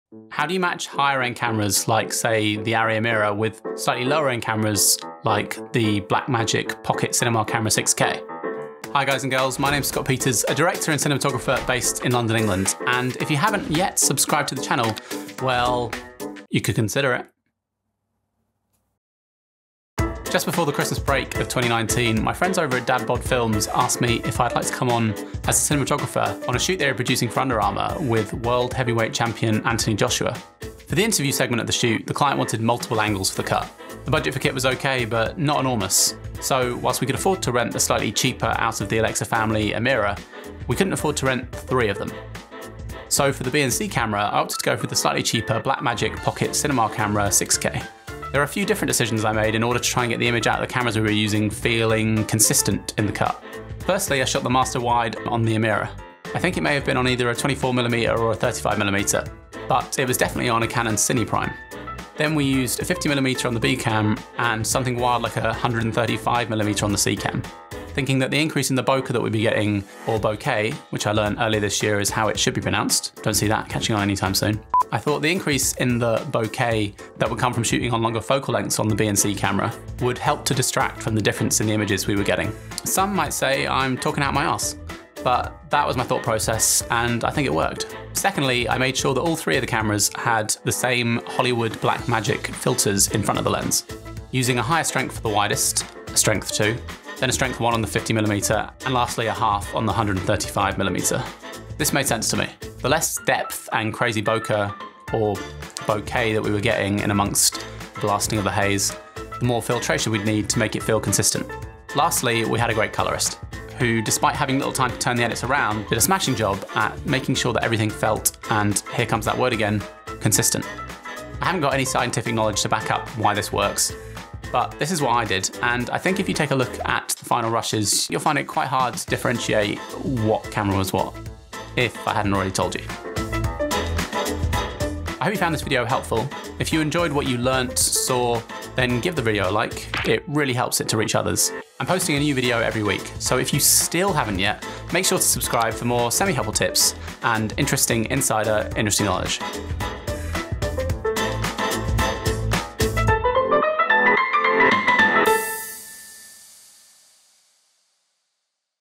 How to Match High End Cinema Cameras with Lower End Cameras
It's not uncommon to have a B or even a C camera added into the mix last minute, that, due to either budget or availability does not match up very well with the A camera. I faced this challenge on a 3 camera shoot with heavyweight champion Anthony Joshua and Under Armour a couple years back when shooting on the Arri Amira, when told we needed to add a B and a C camera for the interview set up, but had budget constraints we could only afford a Blackmagic Pocket Cinema Camera 6K at best, and this is what I did to make the different brands of camera work together.